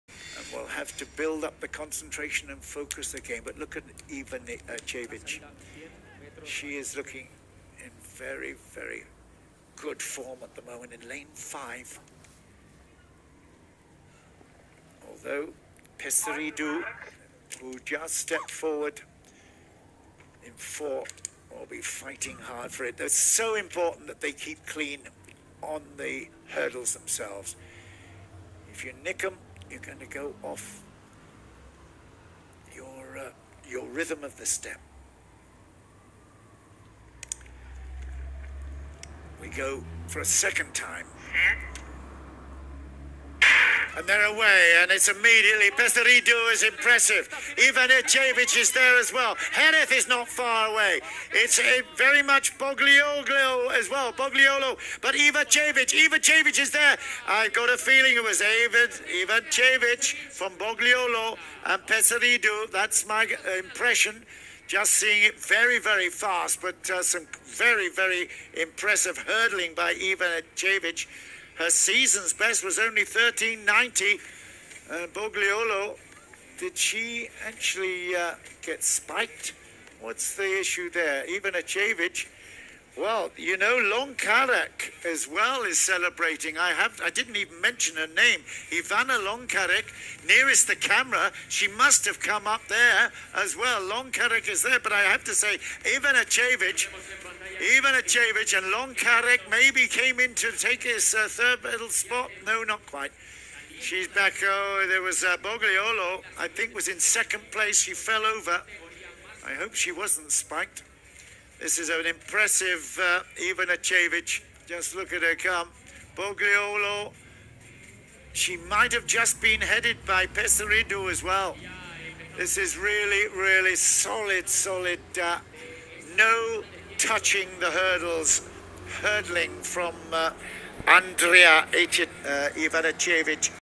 Women's final100m hurdles at Mediterranean Games in Catalonia 2018
100 meters hurdles women athletic's.